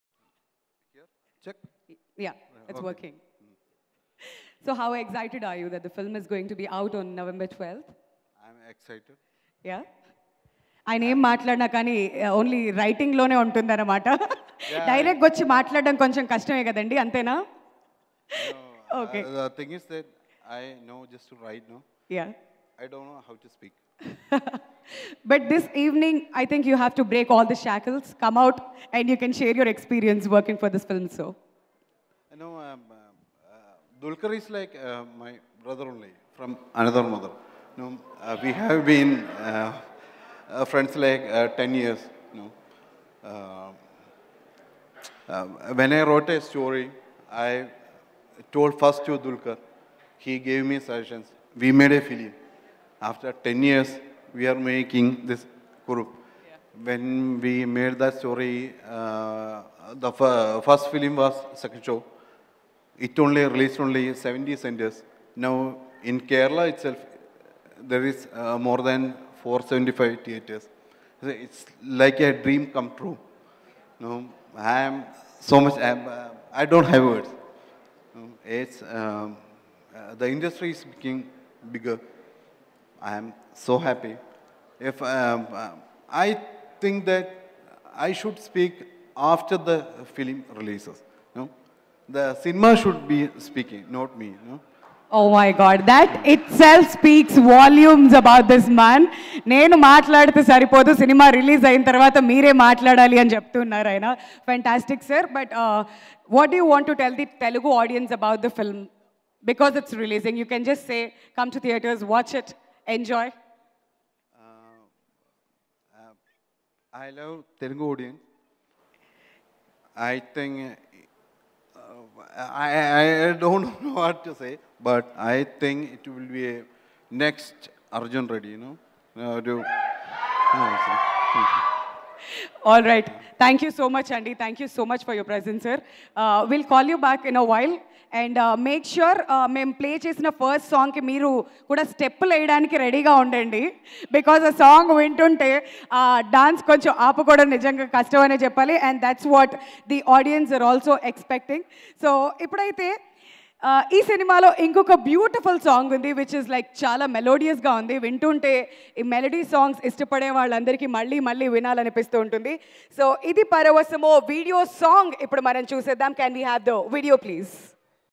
Writer Vini Viswa Lal Speech At KURUP Pre Release Event | Dulquer Salmaan | ZUP TV
Watch Writer Vini Viswa Lal Speech At KURUP Pre Release Event | Dulquer Salmaan | ZUP TV